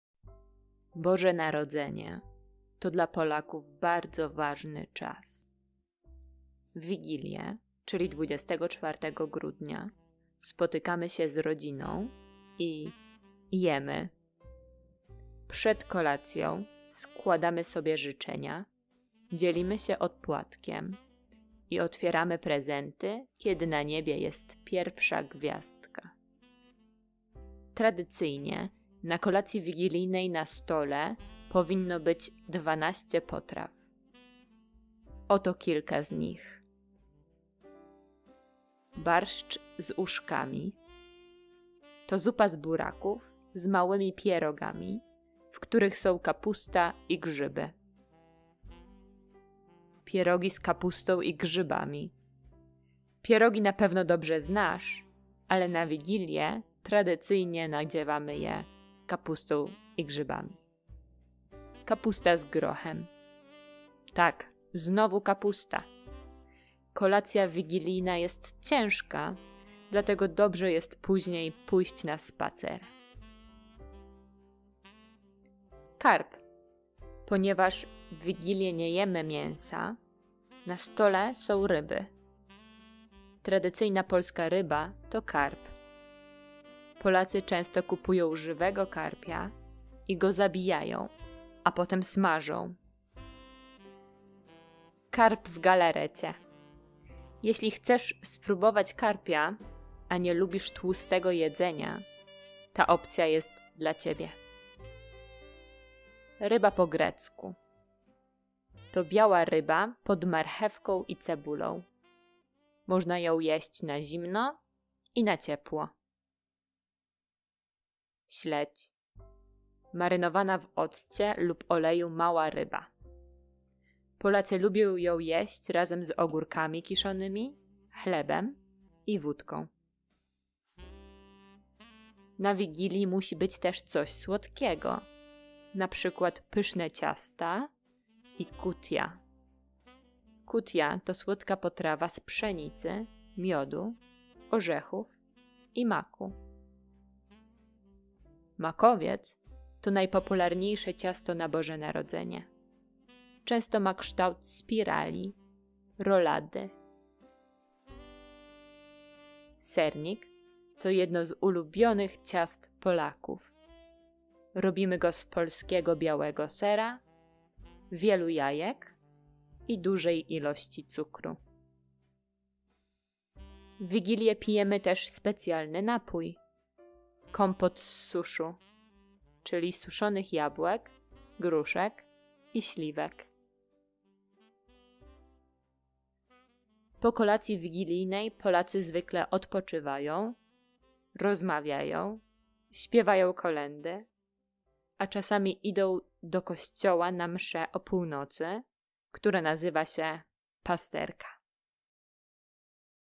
Polish Christmas Dishes - Polskie Potrawy Wigilijne
Learn how to pronounce the names of these Polish Christmas' Eve dishes and practice your listening. in tab Studying Materials.